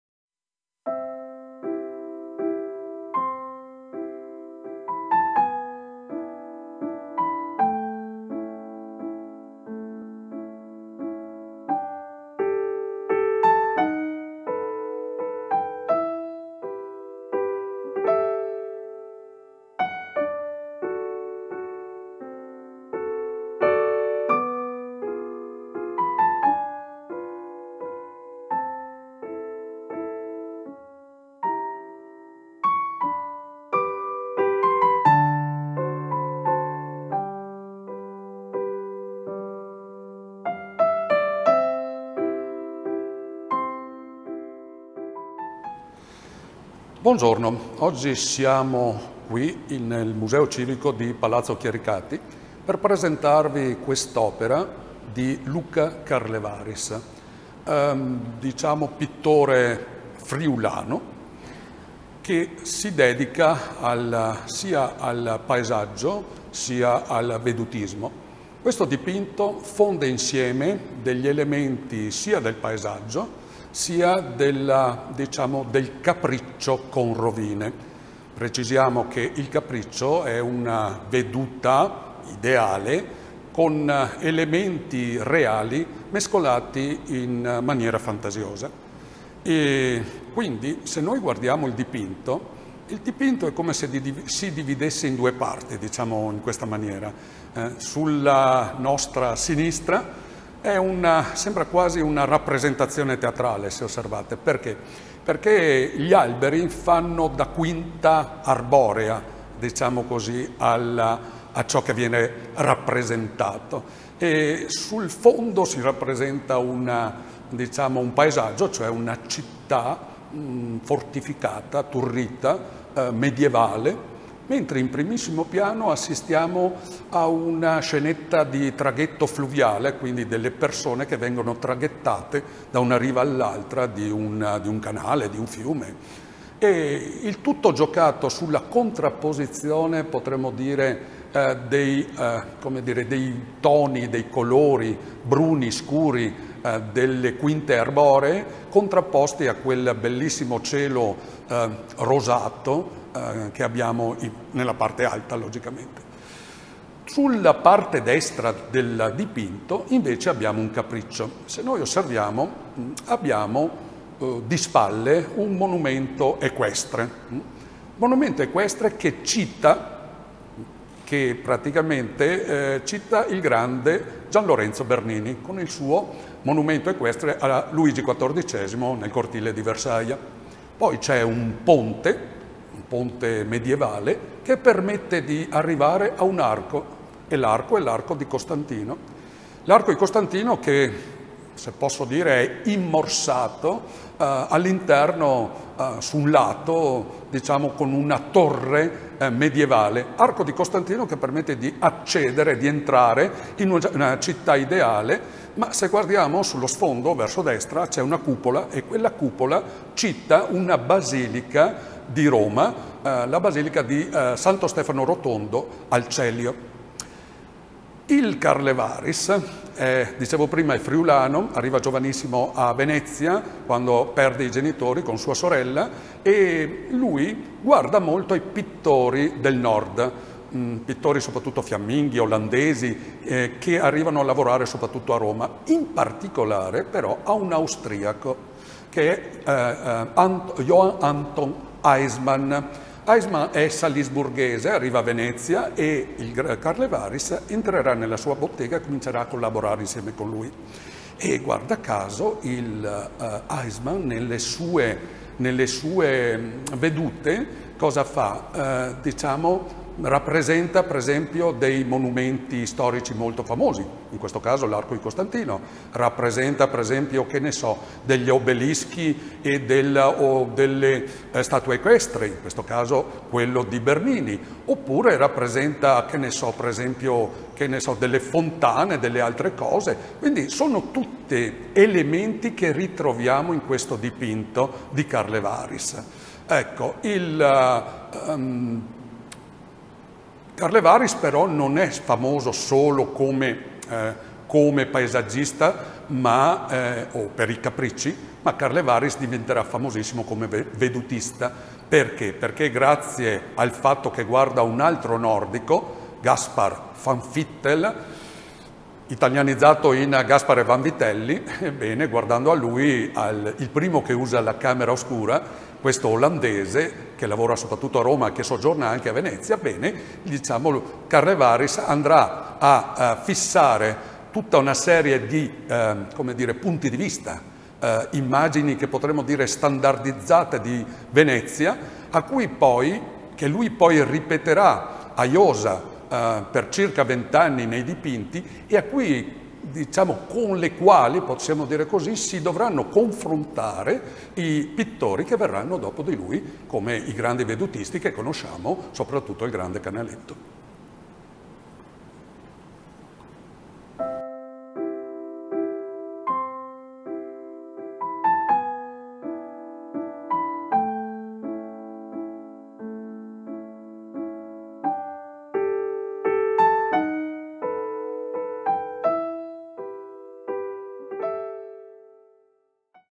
"Pillole d'arte": paesaggio di Luca Carlevarijs a Palazzo Chiericati
La serie di video "Pillole d'arte" nasce da una collaborazione tra i Musei Civici di Vicenza, l'assessorato alla cultura e le Guide turistiche abilitate Vicenza.
In ogni "pillola" viene descritta un'opera d'arte, per scoprire a poco a poco il ricco patrimonio museale della città di Vicenza.

In questo video la guida turistica Simone Massaro descrive l'opera di  Luca Carlevarijs "Paesaggio con arco trionfale e monumento equestre" del 1721 conservata a Palazzo Chiericati.
La veduta fonde elementi del paesaggio con quelli del capriccio con rovine. Il monumento equestre sull'alto basamento  è un omaggio a Gian Lorenzo Bernini. Accanto un ponte medioevale conduce all'Arco di Costantino.